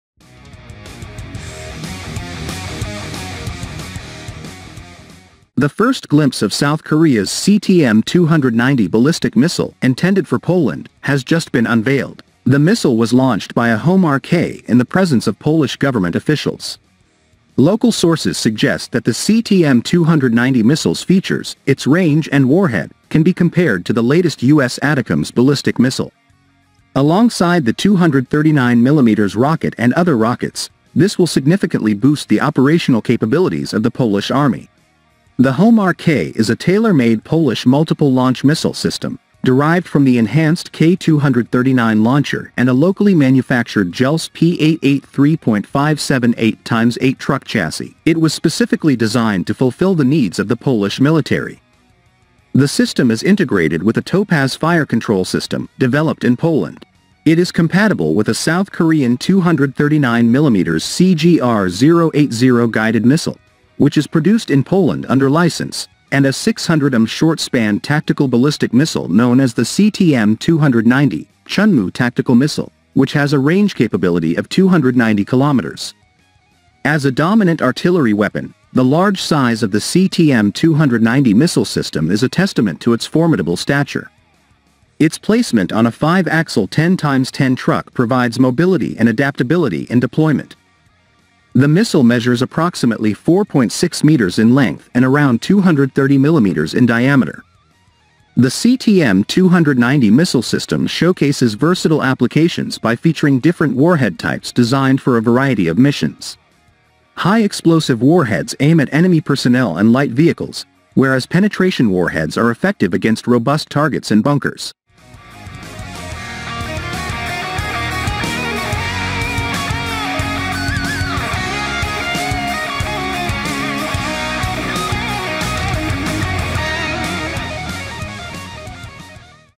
CTM-290 missile launched from Poland's Homar-K for the first time
The first glimpse of South Korea’s CTM-290 ballistic missile, intended for Poland, has just been unveiled. The missile was launched by a Homar-K in the presence of Polish government officials. Local sources suggest that the CTM-290 missile’s features – its range and warhead – can be compared to the latest US ATACMS ballistic missile. Alongside the 239 mm rocket and other rockets, this will significantly boost the operational capabilities of the Polish army.

The Homar-K is a tailor-made Polish multiple-launch missile system, derived from the enhanced K239 launcher and a locally manufactured Jelcz P883.57 8×8 truck chassis. It was specifically designed to fulfill the needs of the Polish military.

The system is integrated with a TOPAZ fire control system, developed in Poland. It is compatible with a South Korean 239 mm CGR080 guided missile, which is produced in Poland under license, and a 600 mm short-span tactical ballistic missile known as the CTM-290 [Chunmoo Tactical Missile], which has a range capability of 290 km.